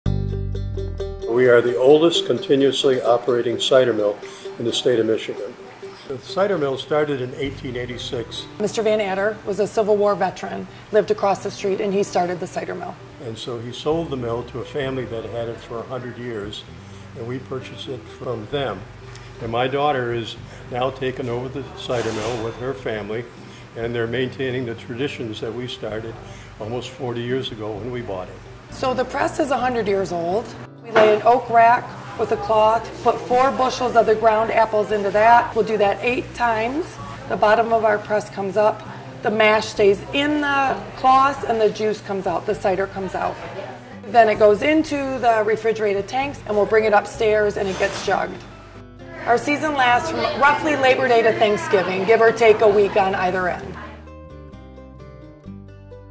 Dexter's Cider Mill - pressing cider the same way since 1886
The Dexter Cider mill is the oldest continuously operational cider mill in Michigan. It was opened in 1886 and still presses cider today.